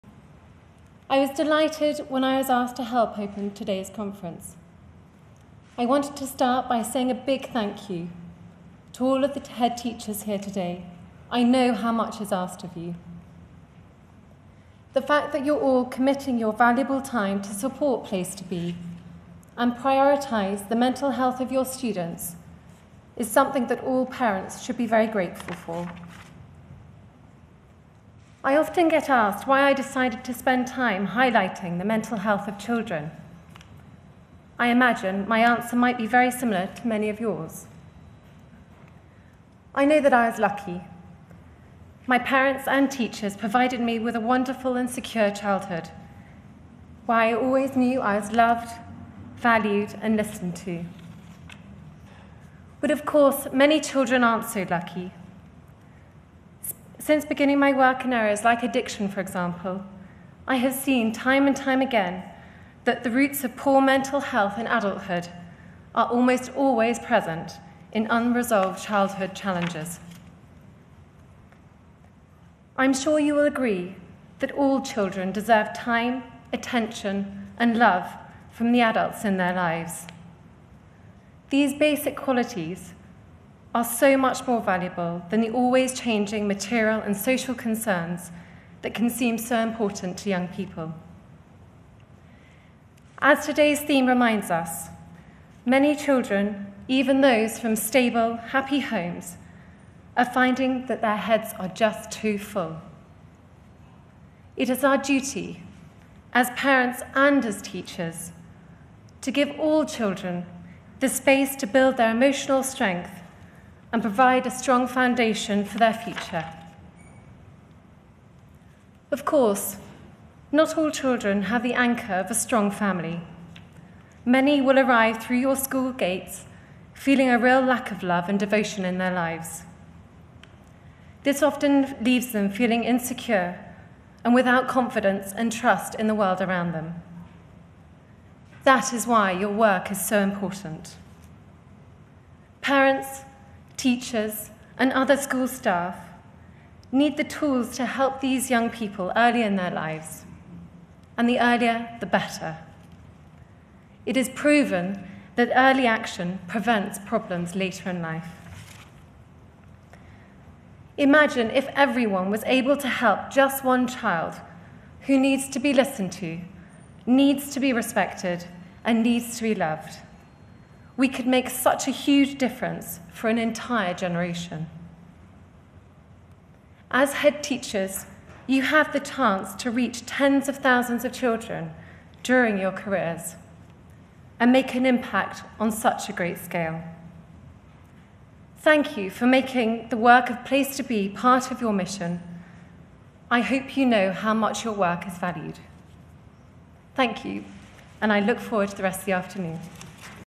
Kate Middleton delivers speech on children's mental health - Daily Mail
The mother-of-two was at the Place2Be Headteacher Conference in central London, which aims to improve the understanding surrounding young people's mental health. Kate, who wore an old wardrobe favourite in the form of a £995 dress, intends to highlight the need to intervene with vulnerable young people as early as possible in order to prevent problems in later life. Aides said the address, which she will make at the Place2be Headteacher Conference in London, was &#8216;very much her own&#8217;.